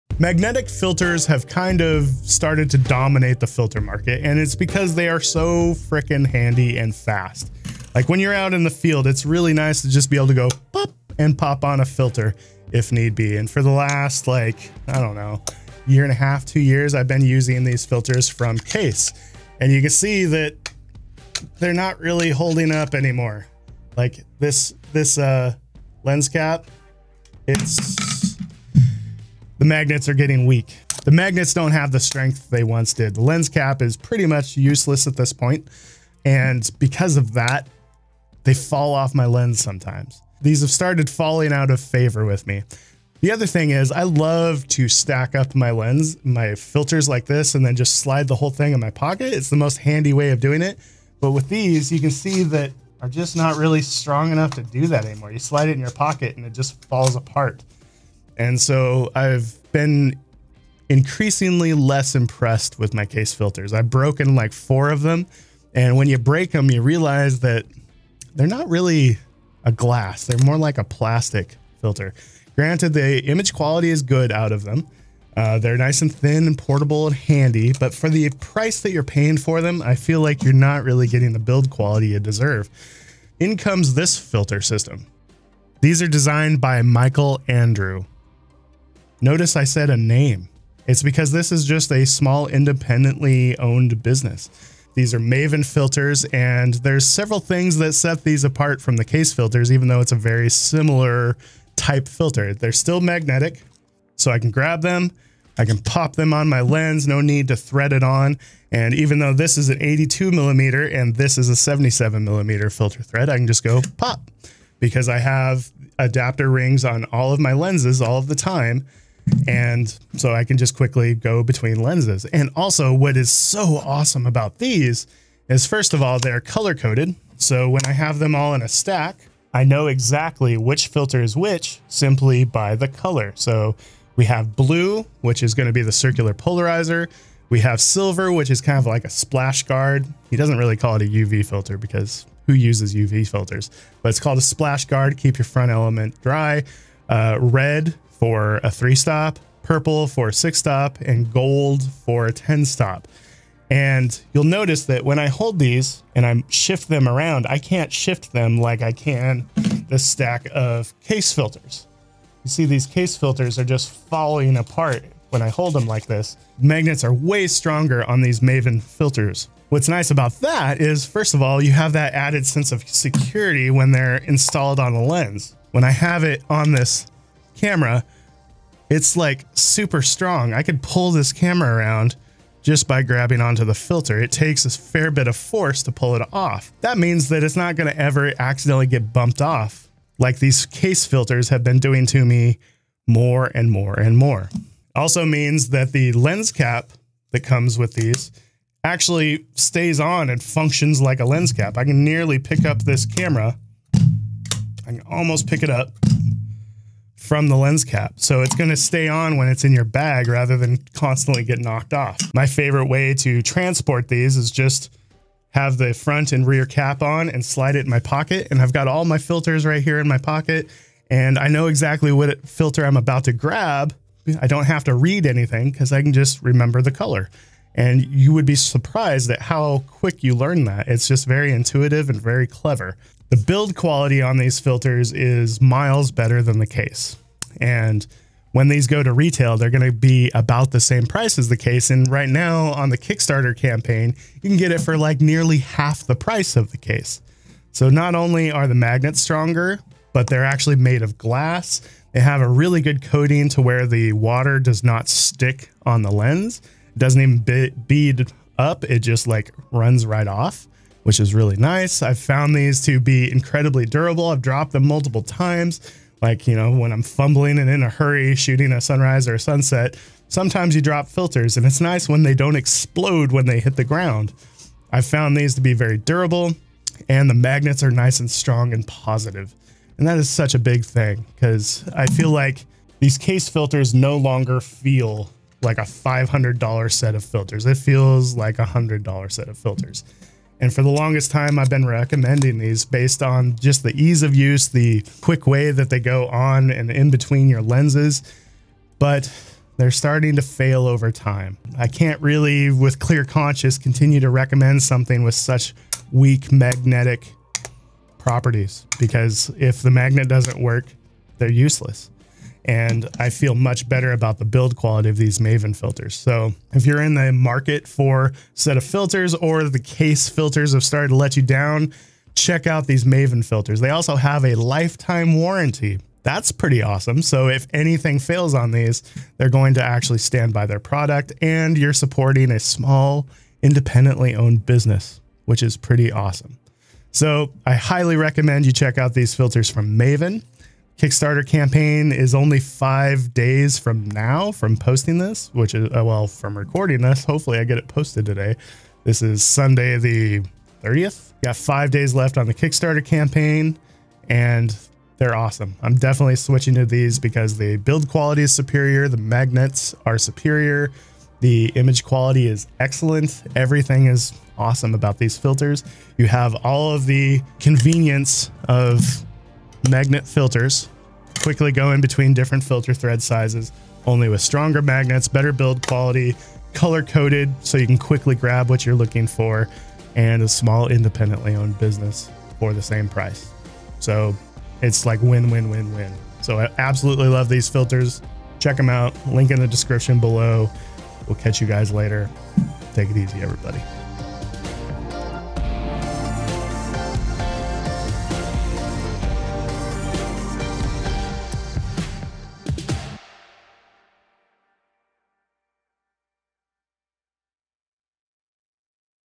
A Better Magnetic Filter system // Maven Magnetic Filters
It was time... my previous filter system had let me down for the last time... dropped filters, easily broken filters and Magnets that couldn't hold fast to a lens.  These new Maven filters are not only built better, with better materials, but the magnets are strong and positive, color coded for ease of use, the same price once they go to retail... but they are independently owned... The color coded design makes these fast and easy to grab the filter you need.  In my opinion they are a better filter  in every way.